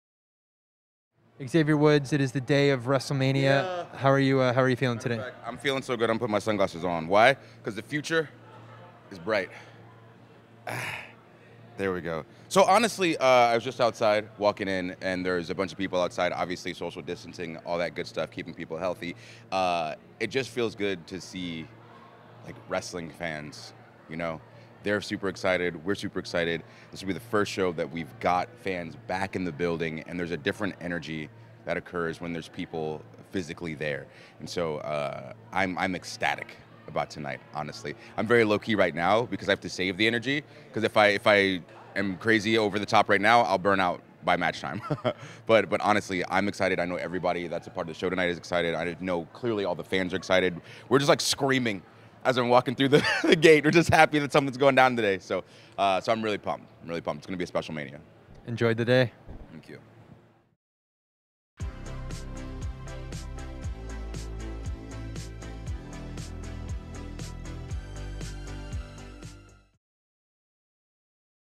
Xavier Woods feels good to see WWE Universe: WrestleMania 37 Exclusive, April 10, 2021
Upon his entrance to Raymond James Stadium, Xavier Woods is already feeling the energy from the fans. Catch WWE action on Peacock, WWE Network, FOX, USA Network, Sony India and more.
Follow WWE on YouTube for more exciting action!